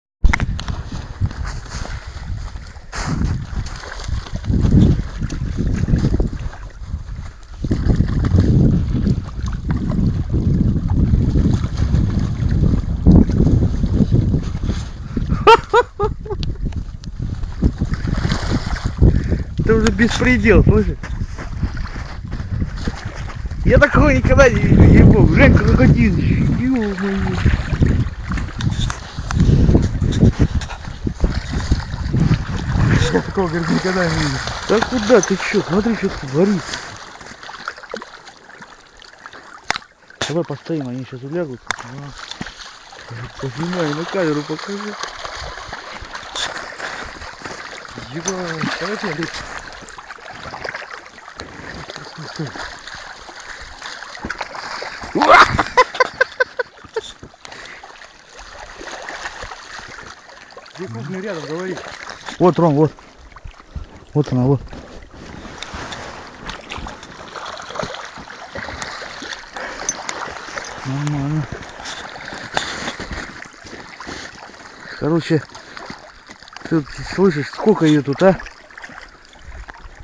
Hand Fishing
How to catch many fish without a rod.
Pêche à la main : comment prendre beaucoup de poisson sans canne à pêche.